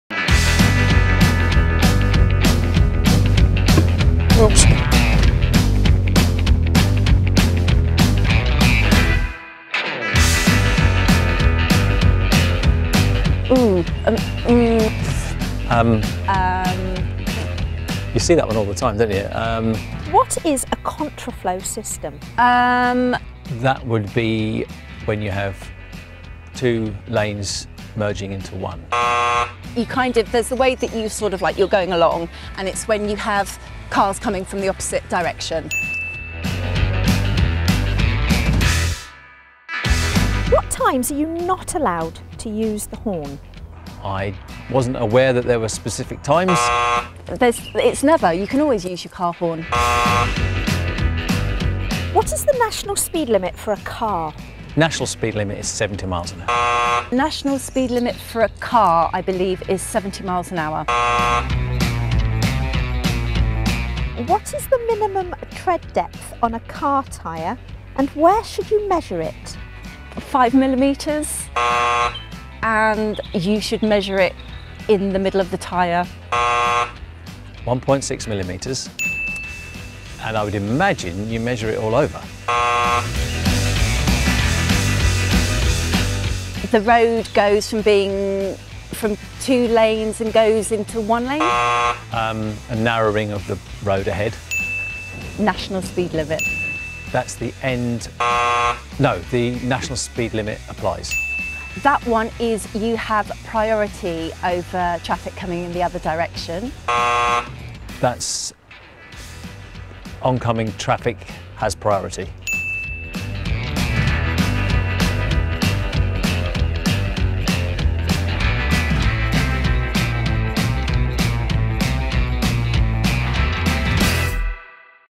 Would you pass a road rules quiz?
Two experienced drivers do their best to answer a quiz on basic Highway Code knowledge. Their best is just not good enough.